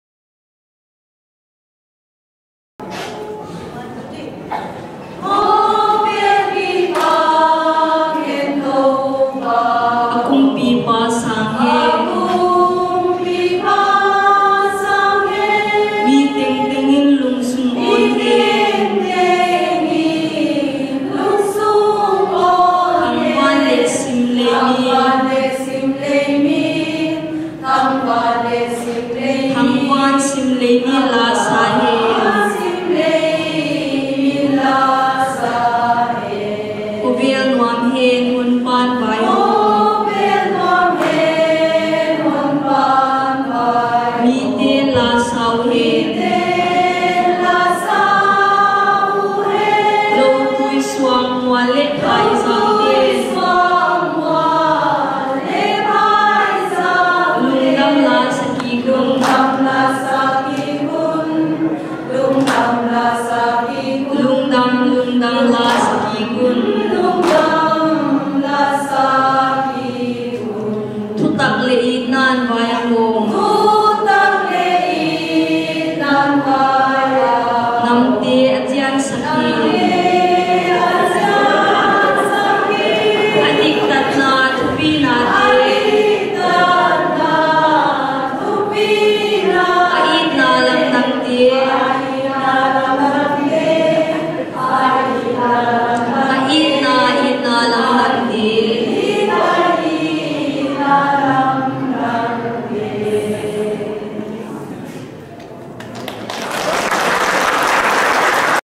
Special Number by BDF
Action song presentation by Baptist Dorcas Fellowship, EBCC Central Delhi.
Christmas 2018 celebrations, Community Centre, Timarpur, Delhi